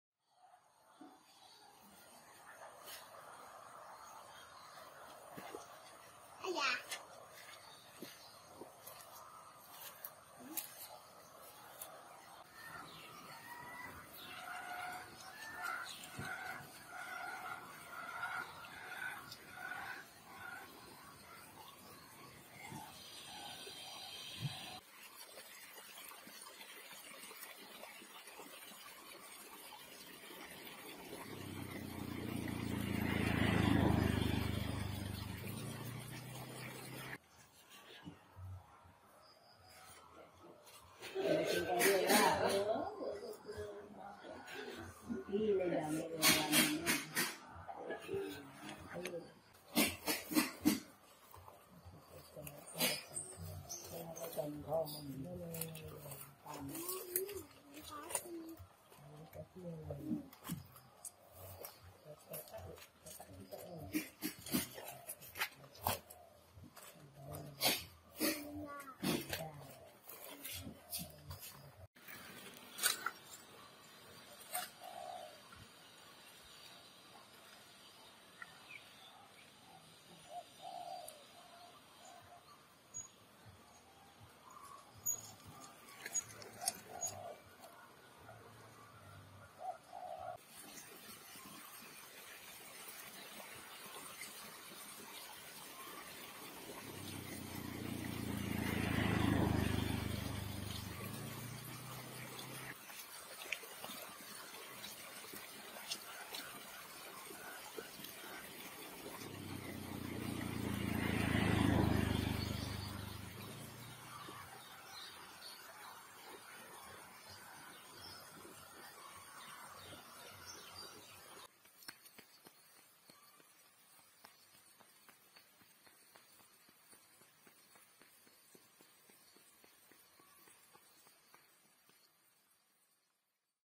KIDS FUN-ACTIVITIES | JAVANESE VILLAGE YOGYAKARTA INDONESIA
@IndoLens - This is a village in Bantul Yogyakarta Indonesia 🇮🇩